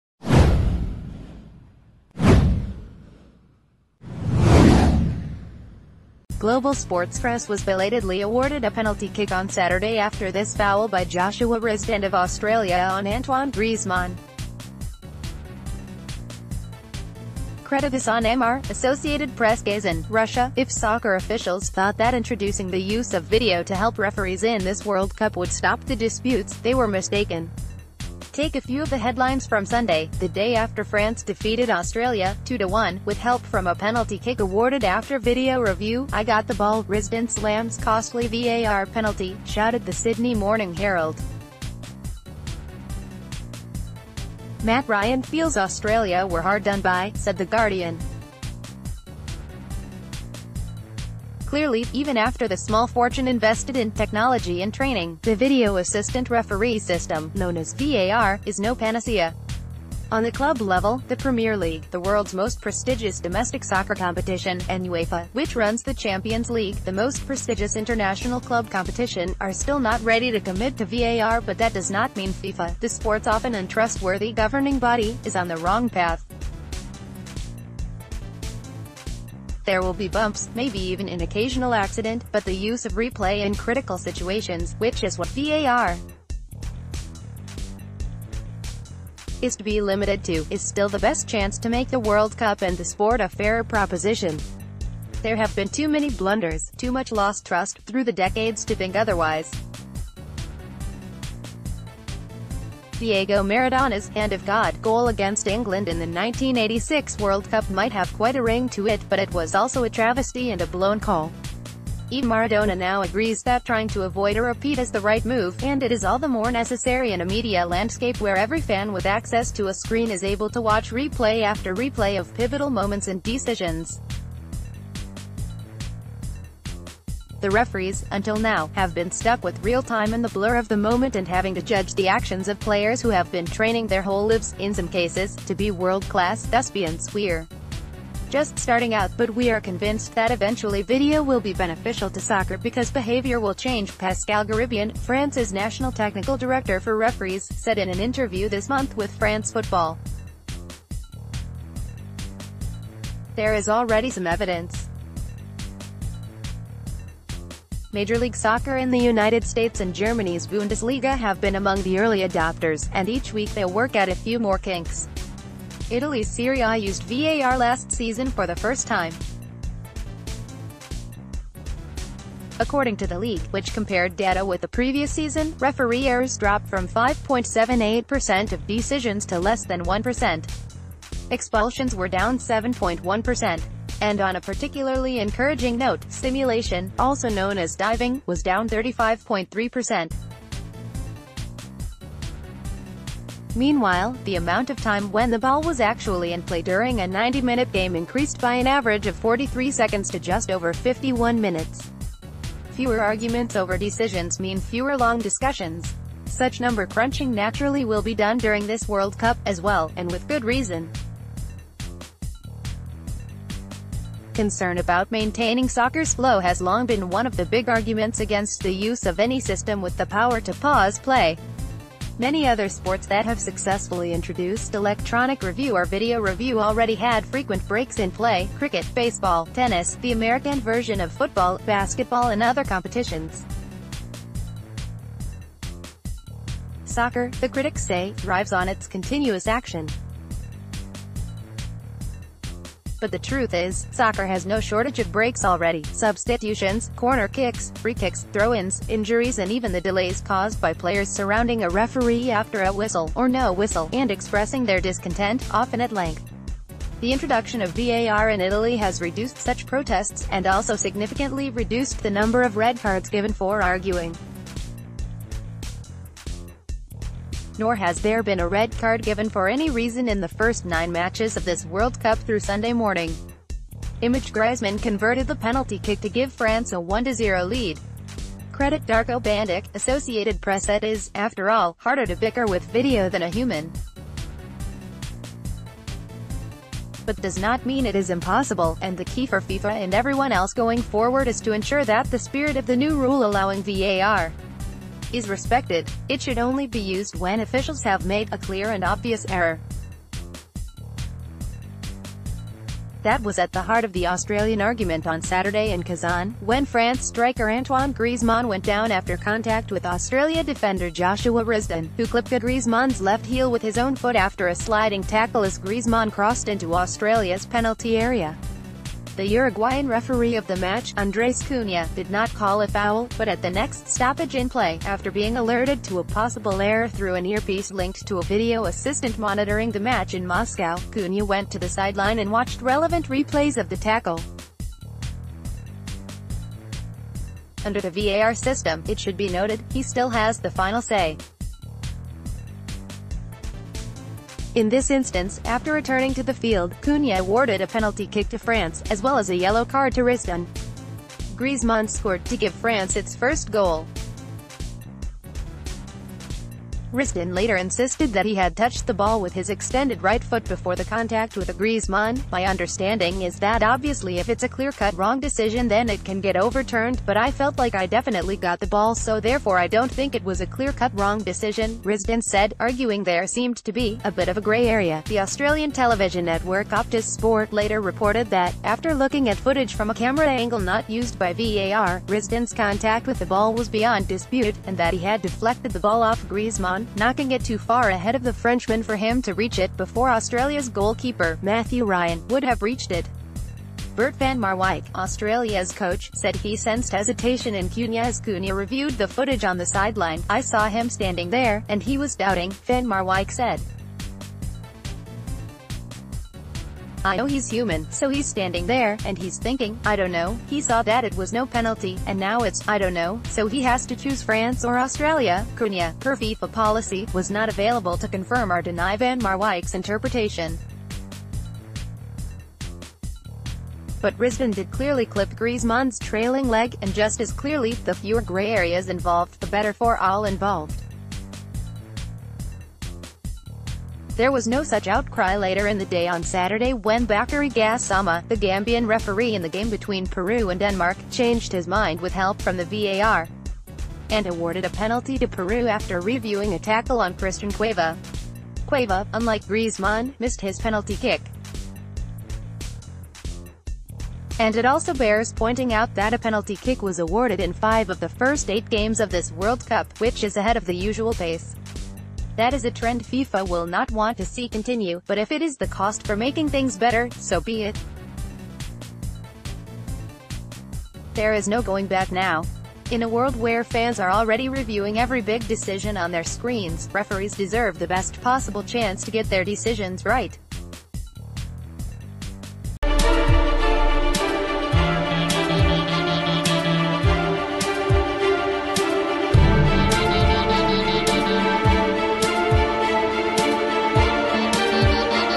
Video Review Will Make the World Cup Better, but Not Quieter
Global SportsFrance was belatedly awarded a penalty kick on Saturday after this foul by Joshua Risdon of Australia on Antoine Griezmann. CreditHassan Ammar/Associated PressKAZAN, Russia — If soccer officials thought that introducing the use of video to help referees in this World Cup would stop the disputes, they were mistaken. Take a few of the headlines from Sunday, the day after France defeated Australia, 2-1, with help from a penalty kick awarded after video review:“‘I Got the Ball’: Risdon ...